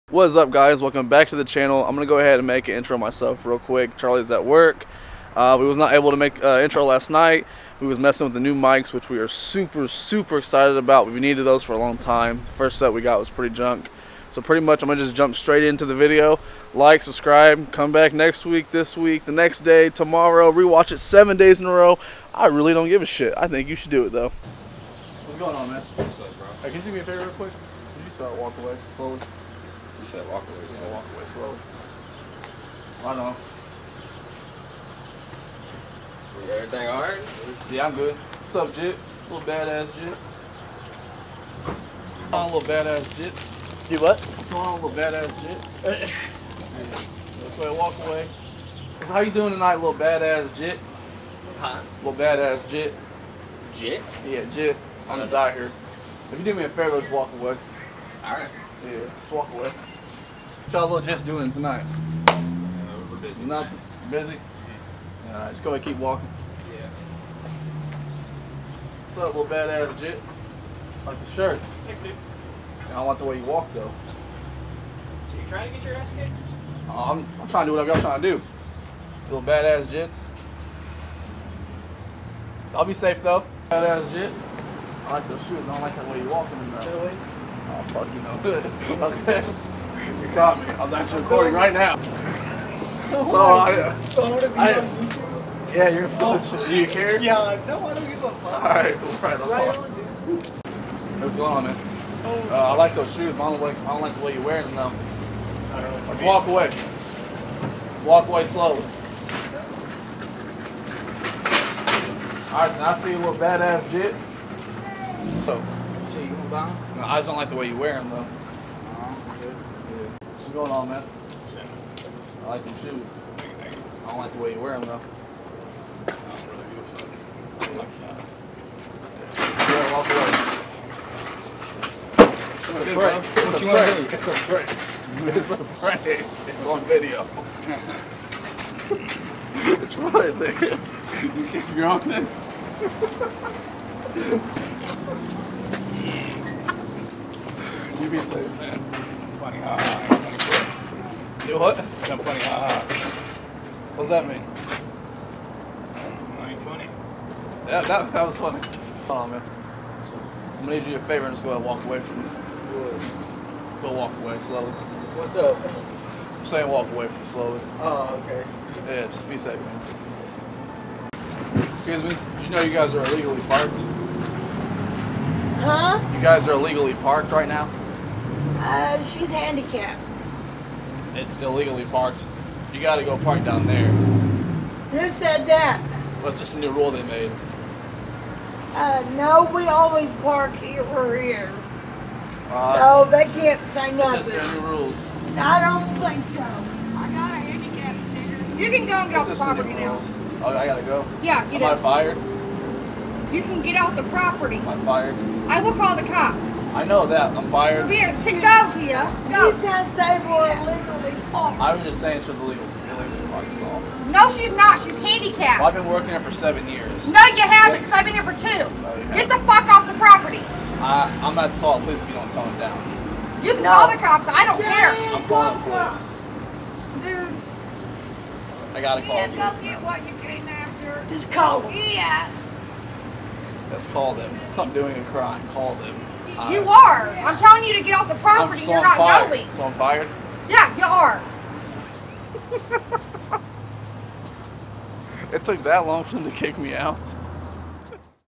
GUY TRIES TO FIGHT ME AND LADY TRIES TO CALL COPS ON ME!!
the public gets irritated with me some times.